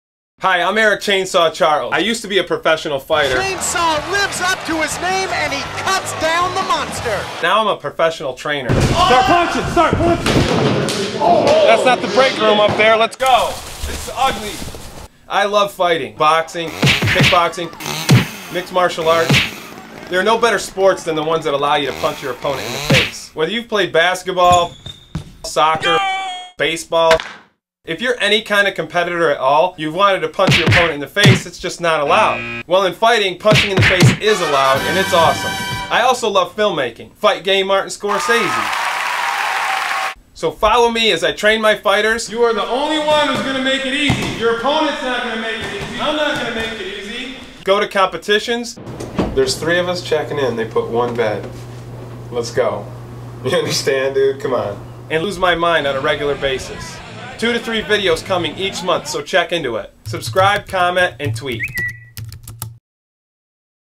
Welcome to Fighting | Fight Game Martin Scorcese by Erik "Chainsaw" Charles
My name is Erik "Chainsaw" Charles.  I used to be a professional fighter.  Now, I'm a professional trainer.  I love fighting and I love film making.  Follow me as I train my fighters, go to competitions, and lose my mind on a regular basis.  Let me know what you think.